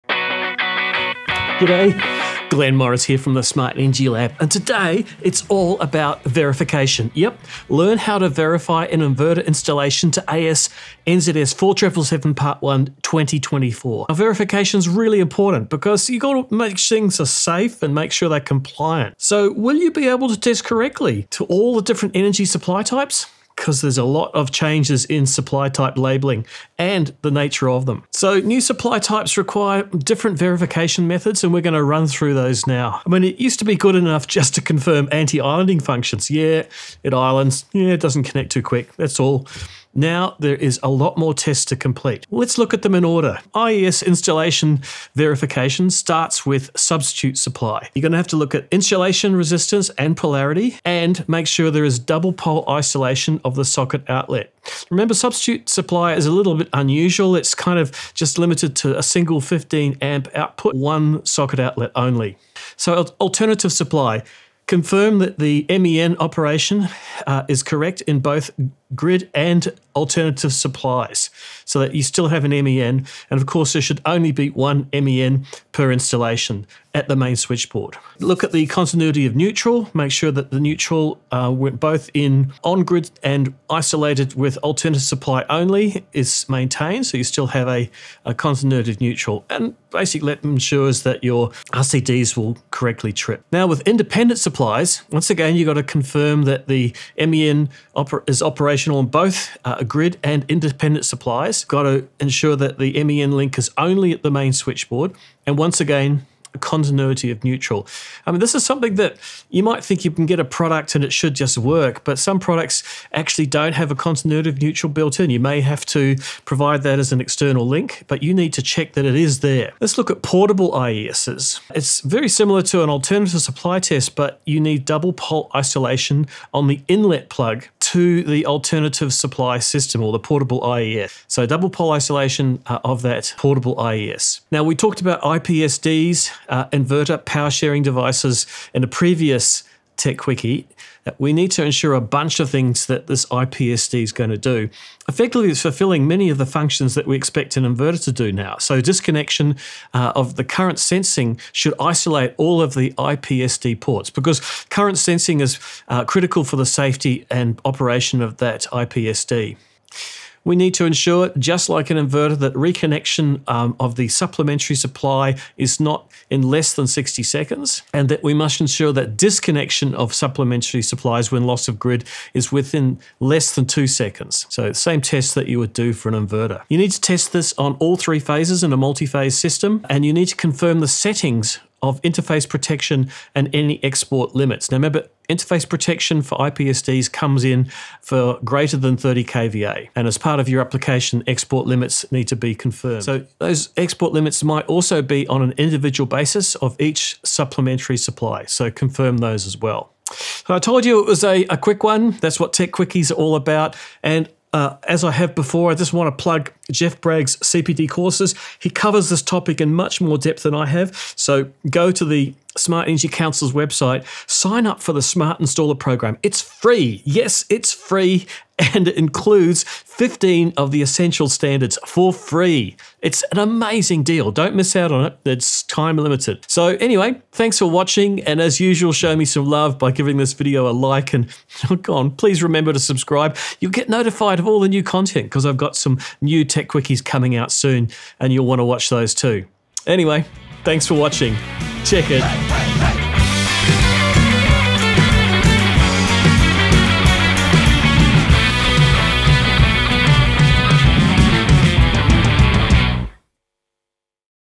Tech Quickie #4 on changes to AS/NZS 4777.1
IES Installation Verification
- Substitute Supply
 - insulation resistance and polarity
 - double pole isolation of socket outlet
- Alternative Supply
 - confirm MEN operational in both grid and alternative supplies
 - MEN link only at MSB
 - continuity of Neutral
- Independent Supply
 - confirm MEN operational in both grid and independent supplies
 - continuity of Neutral
- Portable IES
 - same as alternative supply test
 - double pole isolation of inlet plug
- IPSD
 - disconnection of current sensing isolates all IPSD ports
 - reconnection of supplementary supply greater than 60 secs
 - disconnection of supplementary supply less than 2 secs
 - test done on all three phases
 - confirm settings of interface protection and any export limits
 - individual export limits of supplementary supplies confirmed